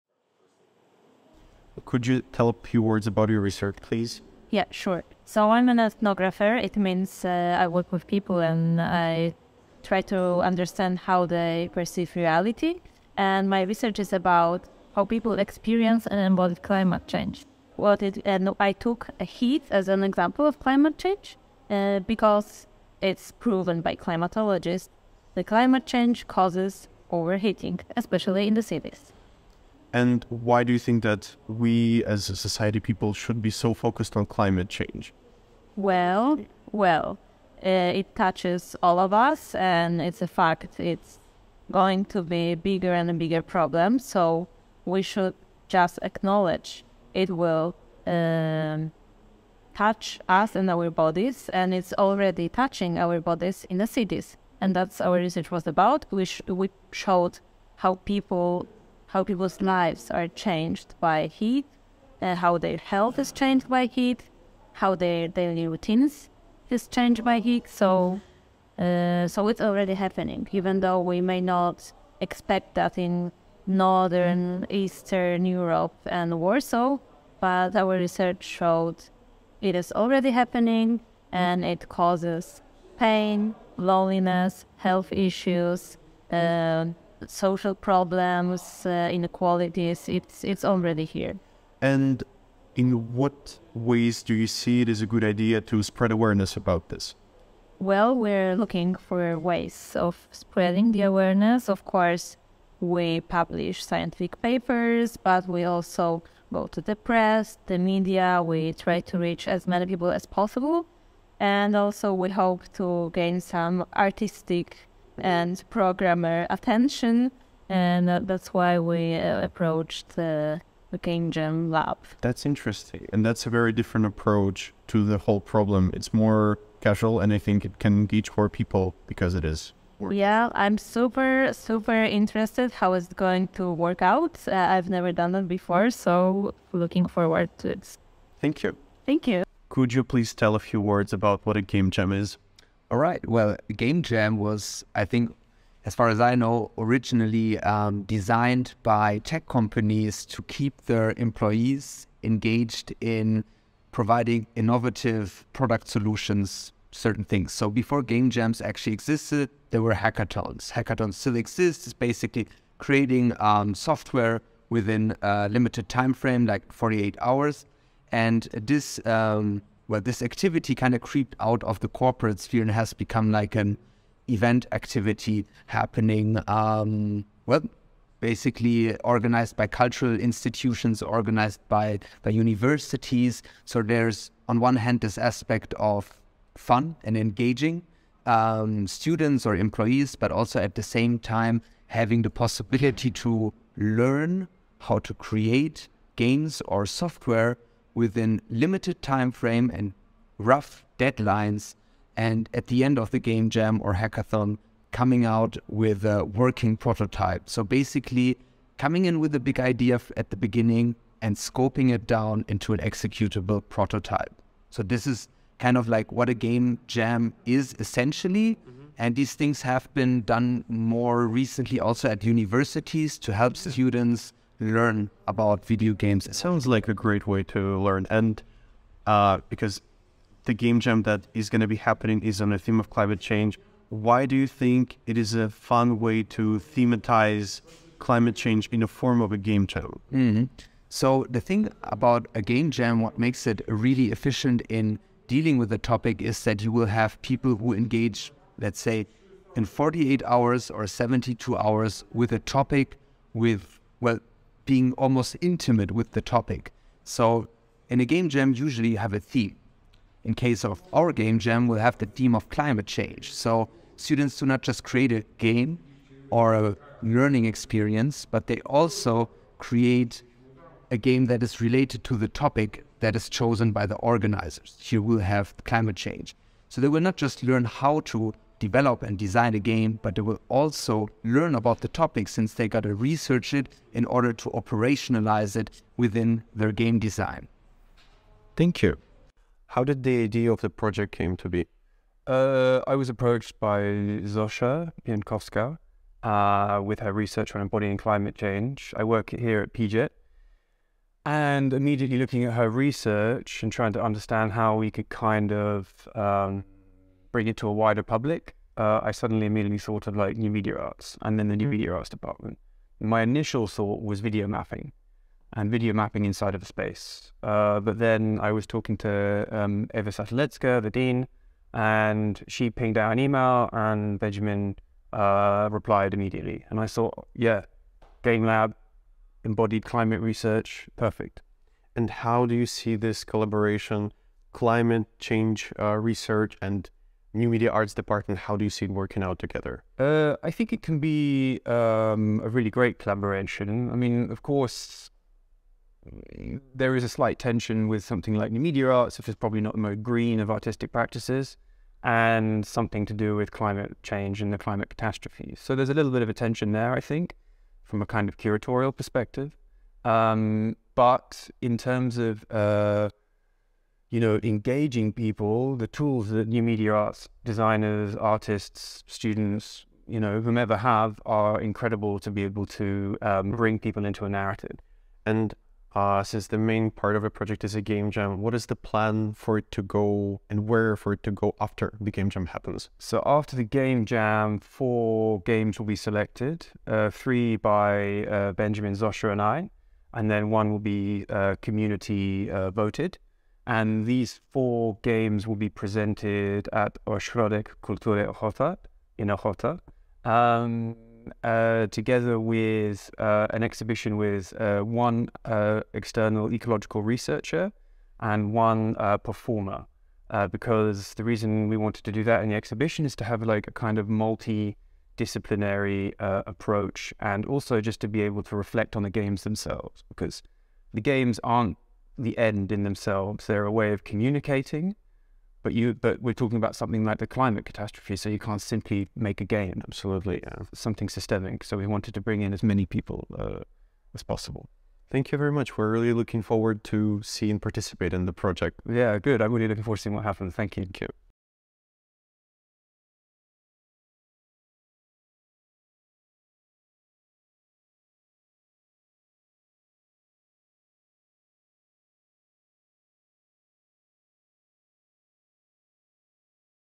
EmCliC GAME JAM 23: Zofia Bienkowska, Benjamin Hanussek, Josh Plough | Hotspot Interview
Episode 6.
Participate in a unique game design challenge that aims to raise awareness of the dangers of Heat Stress caused by Climate Change and Urban Overheating. With a hybrid format, the event encourages students to create interactive experiences for the wider public, exploring themes such as accessibility, urbanisation, social inequalities, and physical health. Don't miss the chance to showcase your creativity and contribute to an art exhibition on Urban Overheating in Warsaw!

This event has been organised by:
Benjamin Hanussek - Head of PJAIT Game Lab
Zosia Bienkowska - Anthropologist & Social Scientist at the University of Warsaw
Josh Plough - Cultural Manager & Founder of Ziemniaki i Foundation
Karyna Haiduk & Liliia Kravchenko - Itch.io Design & Visual Identity
Dmitry Pisarchik, Ivan Tsimchyshyn & Kirill Varmalov - Video Production
Sofiia Orlovska & Oleksandra Ivushkina-Shmitt - Instagram
Vladlen Zabelin - Promotional Posters
Anastasiia Yurchenko - Discord

Made by: Ivan Tsimchishyn, Dmitry Pisarchik, Kirill Varmalov

2023

00:00 On Climate Change
02:20 On Game Jams
04:50 On the Concept